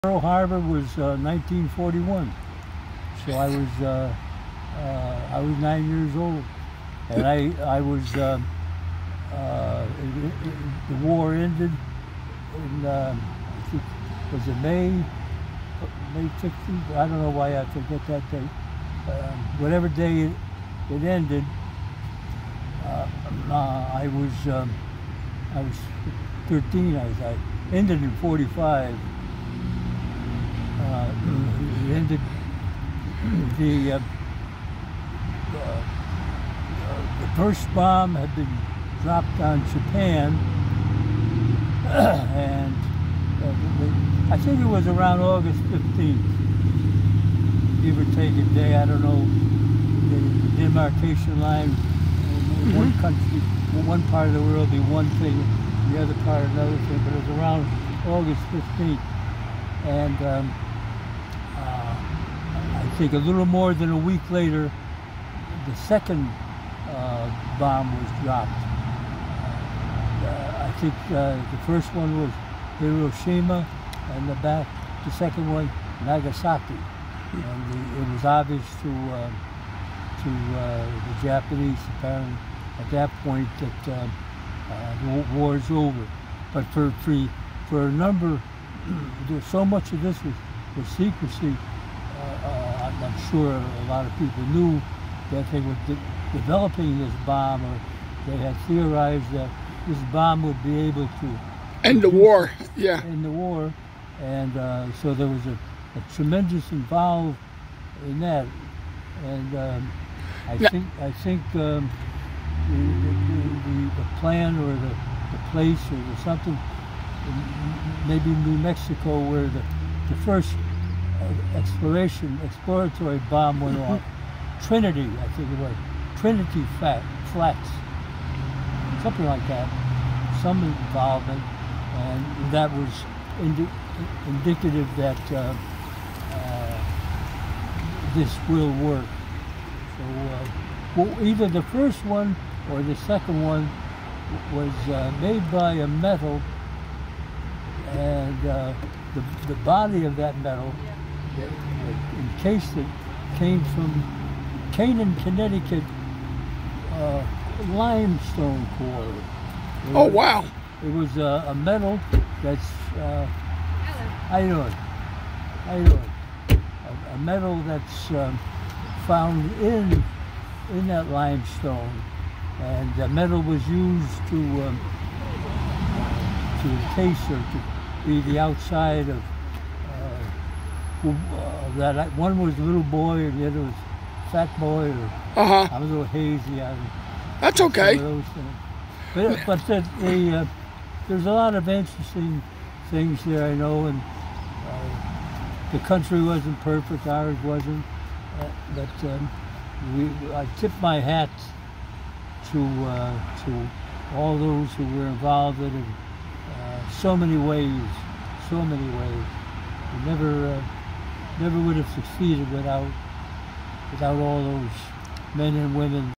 070421 IWO JIMA MEMORIAL, CT(5)
070421 Iwo Jima Memorial, one CT's personal memories of the war.

070421, I had the pleasure of speaking with CT   resident Joe M on growing up during the war. Joe M. was nine years old, when Pearl Habor happened.